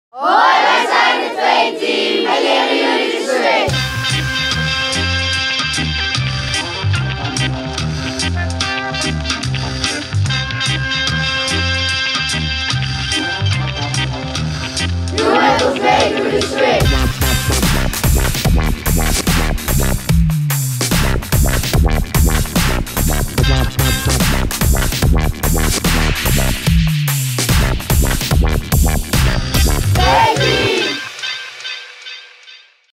Vteam Tutorial Swish
De meeste kinderen kennen 'm al, de meeste volwassenen niet. Ter voorbereiding voor de burgemeesters op de Vierdaagse route hier is: de swish.